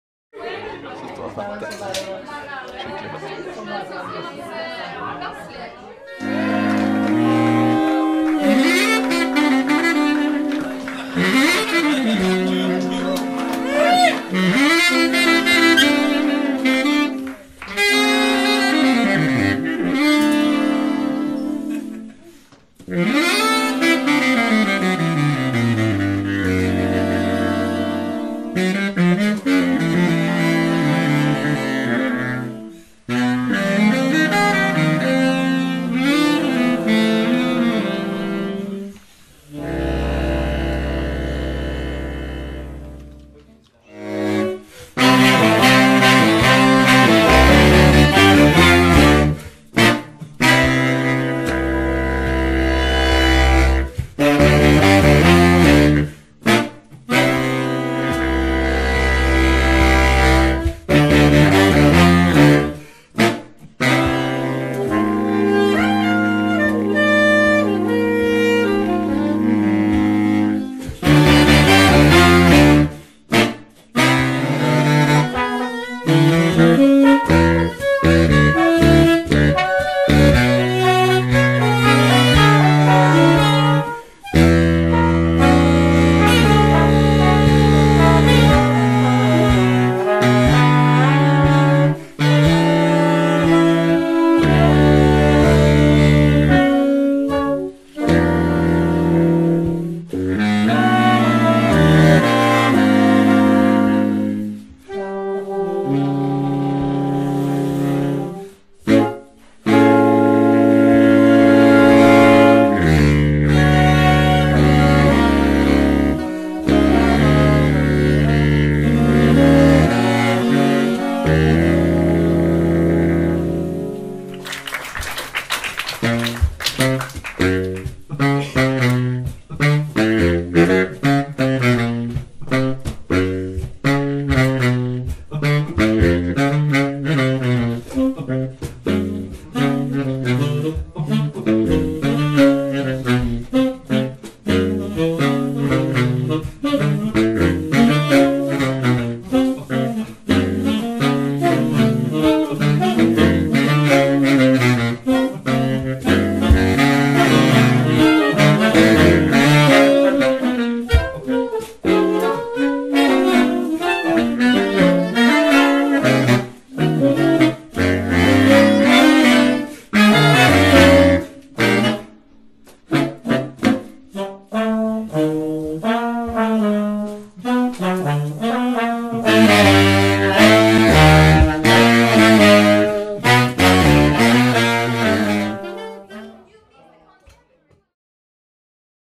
SMEIK
Magnus Murphy Joelson - trombone
Camilla Hole - sopransaksofon
Håkon Hagen Knudsen - altsaksofon
Mathias Hagen - tenorsaksofon
Kristoffer Dokka - barytonsaksofon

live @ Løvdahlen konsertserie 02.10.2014